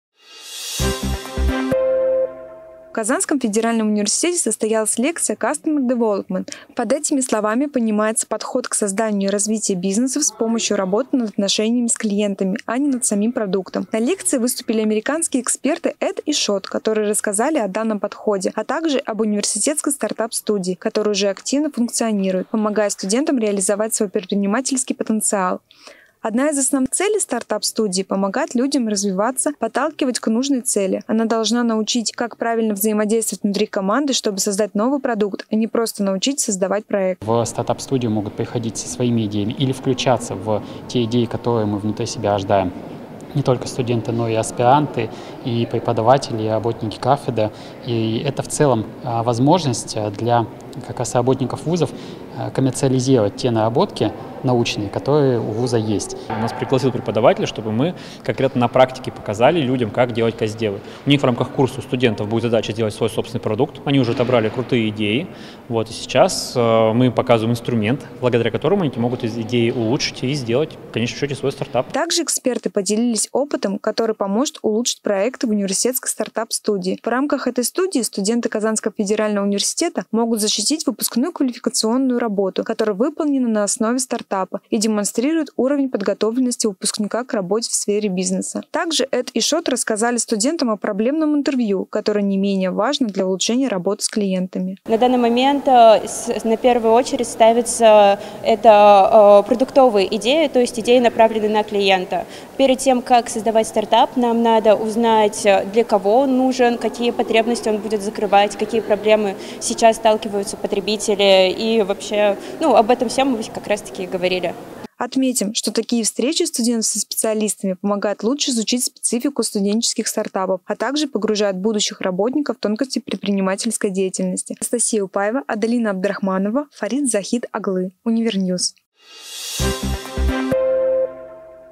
ПРОДАЙ МНЕ ЭТУ РУЧКУ!
Что такое "Customer development" и как стать предпринимателем в стенах университета – расскажет наш корреспондент Анастасия Упаева.